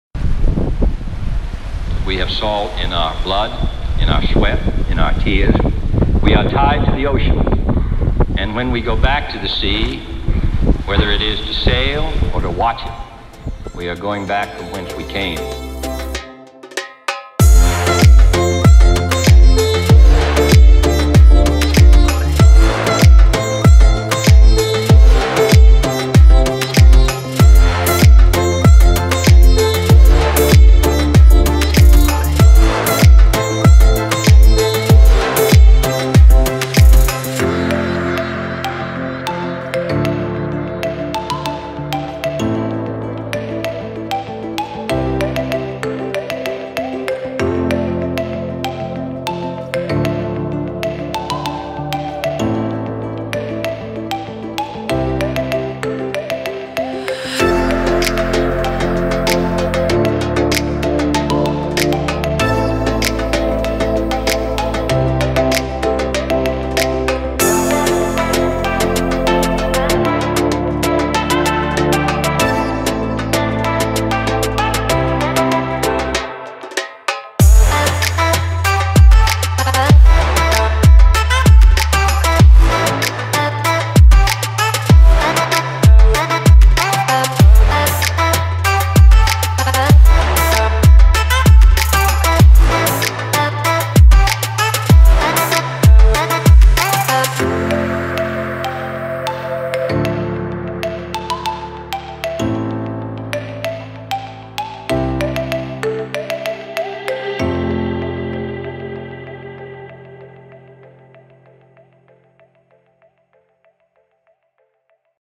Mallorca Sailing
This is the short summary of a beautifull 7 day sailing trip around mallorca. Managed to get some sick shots of Cabrera and the surrounding islands.

Equipment I used:
Gopro 4, DJI Mavic pro